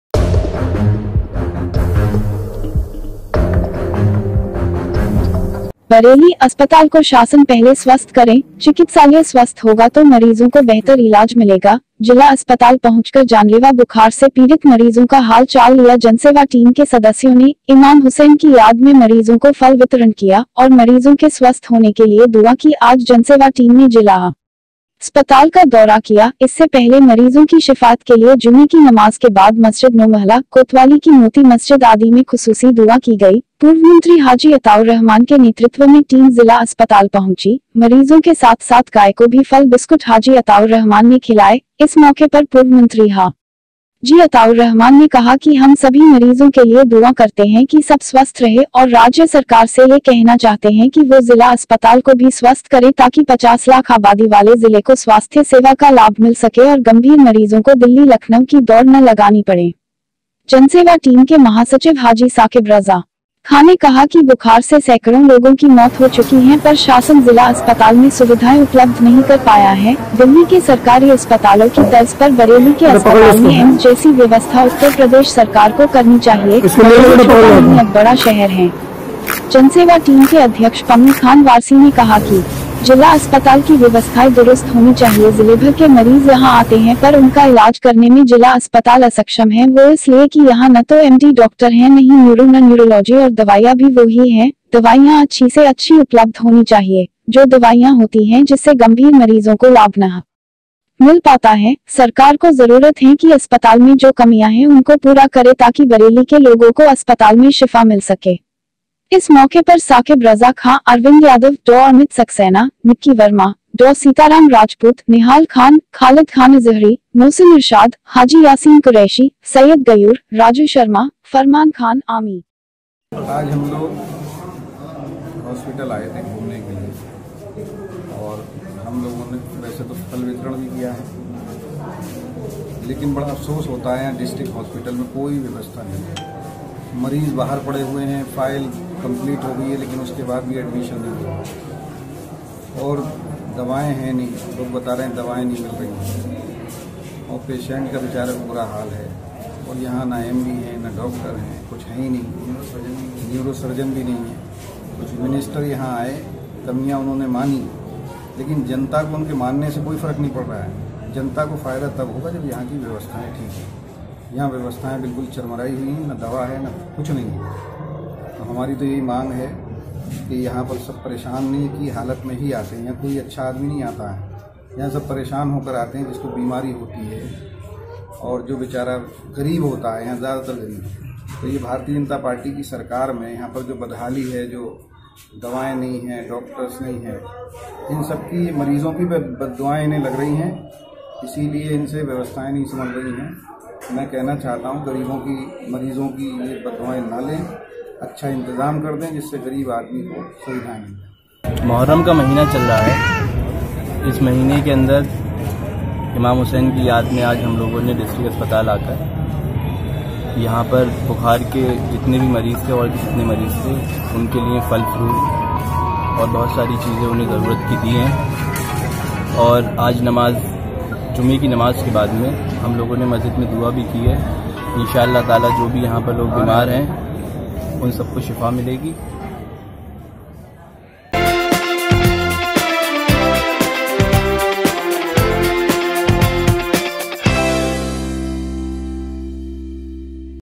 अस्पताल को शासन पहले स्वस्थ करें,चिकित्सालय स्वस्थ होगा तो मरीज़ों को बेहतर ईलाज मिलेगा
जिला अस्पताल पहुँचकर जानलेवा बुखार से पीड़ित मरीज़ों का हाल चाल लिया जनसेवा टीम के सदस्यों ने,

इमाम हुसैन की याद में मरीज़ों को फल वितरण किया,और मरीज़ों के स्वस्थ होने के लिये दुआँ की,

आज जनसेवा टीम ने जिला अस्पताल का दौरा किया,इससे पहले मरीज़ों की शिफ़ाअत के लिए जुमे की नमाज़ के बाद मस्जिद नोमहला, कोतवाली की मोती मस्जिद आदि में खुसूसी दुआँ की गई,

पूर्वमंत्री हाजी अताउर्रहमान के नेतृत्व में टीम ज़िला अस्पताल पहुँची,मरीज़ों के साथ साथ गाय को भी फल बिस्कुट हाजी अताउर्रहमान ने खिलाये,

इस मौके पर पूर्वमंत्री हाजी अताउर्रहमान ने कहा कि हम सभी मरीज़ों के लिए दुआँ करते हैं कि सब स्वस्थ रहे,और राज्य सरकार से ये कहना चाहते हैं कि वो ज़िला अस्पताल को भी स्वस्थ करें ताकि 50 लाख आबादी वाले ज़िले को स्वास्थ्य सेवा का लाभ मिल सके और गम्भीर मरीज़ों को दिल्ली लखनऊ की दौड़ न लगानी पड़े।

जनसेवा टीम के महासचिव हाजी साकिब रज़ा खाँ ने कहा कि बुखार से सैकड़ों लोगों की मौत हो चुकी हैं पर शासन ज़िला अस्पताल में सुविधाऐ उपलब्ध नही कर पाया हैं,दिल्ली के सरकारी अस्पतालो की तर्ज़ पर बरेली के अस्पताल में एम्स जैसी व्यवस्था उत्तर प्रदेश सरकार को करनी चाहिये,बरेली छोटा नही अब बड़ा शहर हैं।

जनसेवा टीम के अध्यक्ष पम्मी खान वारसी ने कहा कि जिला अस्पताल की व्यवस्थाए दुरुस्त होनी चाहिये ज़िलेभर के मरीज़ यहाँ आते हैं पर उनका इलाज करने में जिला अस्पताल असक्षम हैं वो इस लिये की यहाँ न तो एमडी डॉक्टर हैं न ही न्यूरो न  न्यूरोलॉजी और दवाईया भी वो ही हैं,दवाइयां अच्छी से अच्छी उपलब्ध होनी चाहिये,जो दवाइयां होती हैं जिससे गम्भीर मरीज़ों को लाभ नही मिल पाता हैं,सरकार को ज़रूरत हैं कि अस्पताल में जो कमिया हैं उनको पूरा करे ताकि बरेली के लोगो को अस्पताल में शिफ़ा मिल सके।

इस मौके पर साकिब रज़ा खाँ,अरविंद यादव,डॉ अमित सक्सेना,निक्की वर्मा,डॉ सीताराम राजपूत,निहाल खान,खालिद खान अज़हरी,मोहसिन इरशाद,हाजी यासीन क़ुरैशी,सय्यद गयूर,राजू शर्मा,फरमान खान,आमिर उल्लाह आशु आदि शामिल रहे।